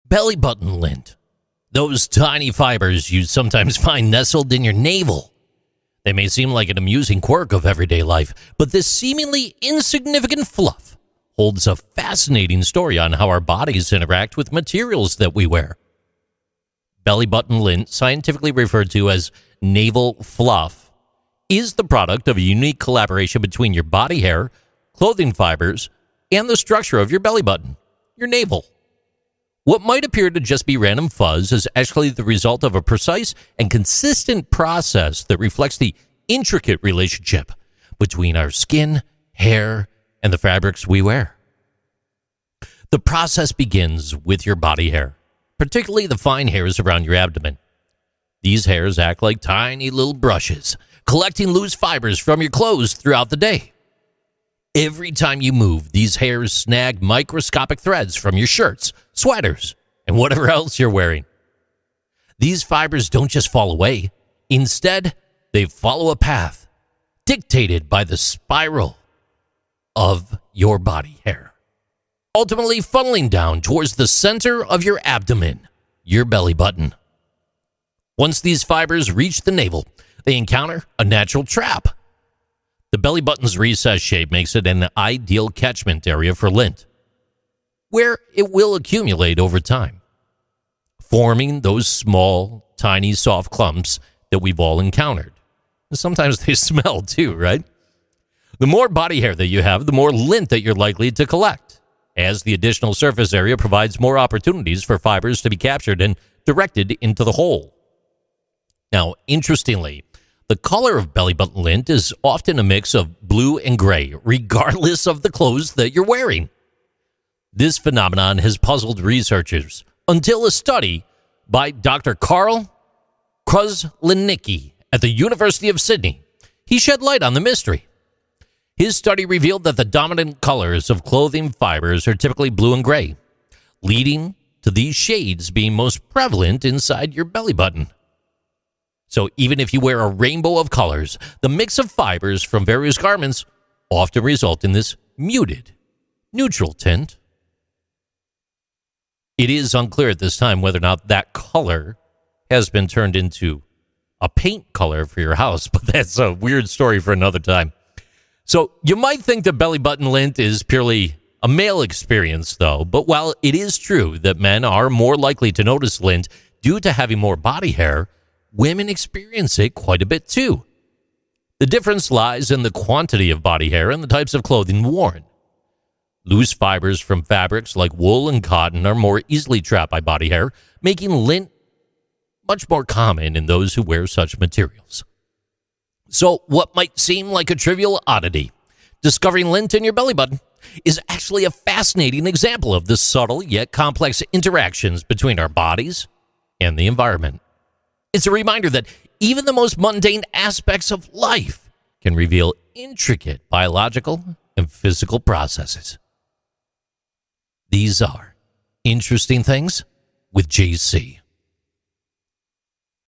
1030: "Belly Button Lint"
Interesting Things with JC #1030: "Belly Button Lint" - What’s the deal with belly button lint?

It might seem trivial, but there's a fascinating science behind how those tiny fibers end up in your navel. 🌟 👩‍🌾

Tune in on @ThePodcastRadio or visit JimConnors.net